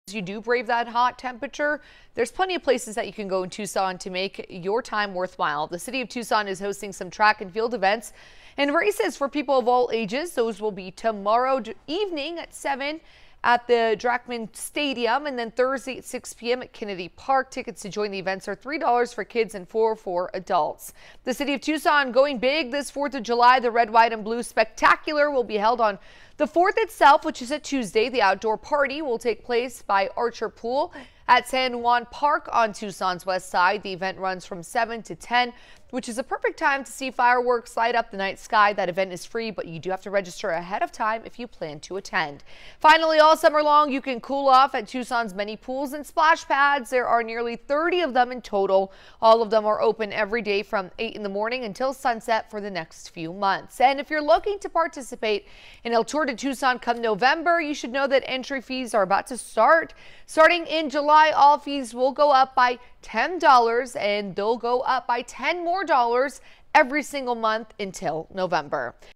Tucson summer events give residents a reason to get outdoors
From track events to signing up for El Tour, the City of Tucson is giving Tucsonans opportunities to get outdoors.